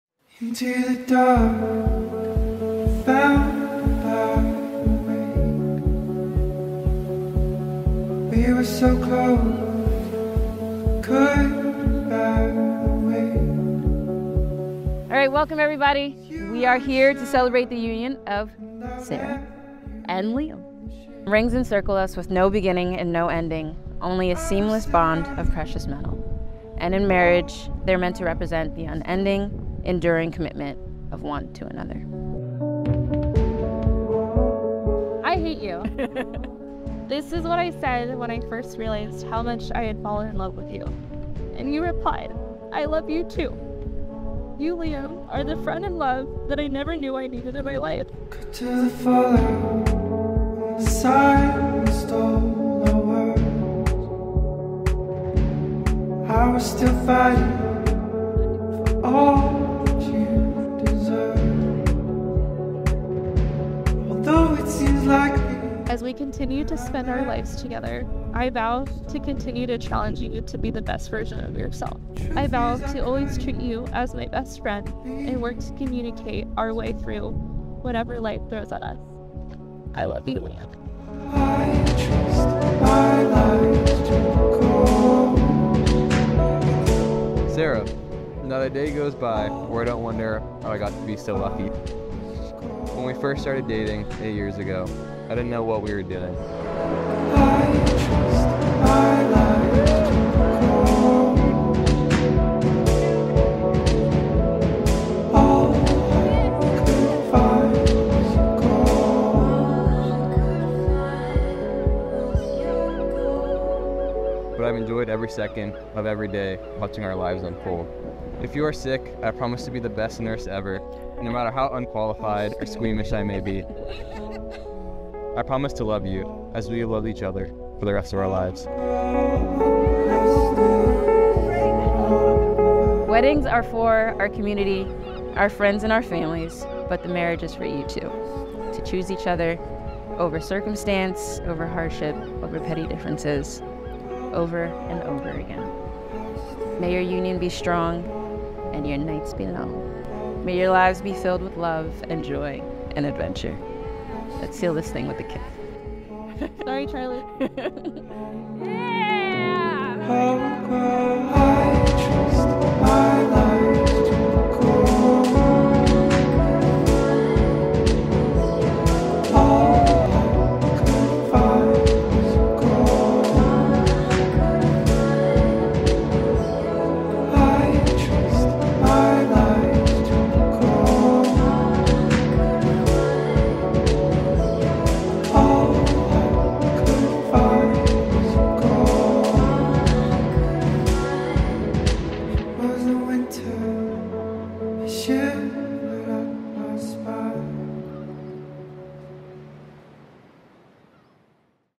The Ultimate San Diego Wedding
What a day! Liam and Sarah celebrated their intimate wedding day along the coast of San Diego with family and friends.

We started the day with them getting ready in Mission Beach and shortly after made our way to OB for street tacos and meeting a few of Sarah's friends who toured us around the life guard station. When you start your day with Tacos and handing out with old lifeguard friends, this truly was the ultimate San Diego wedding.

After a quick pit stop in OB we were off to Sunset Cliffs for one of the most beautiful wedding ceremonies. We are so excited to share the full video, hope you enjoy.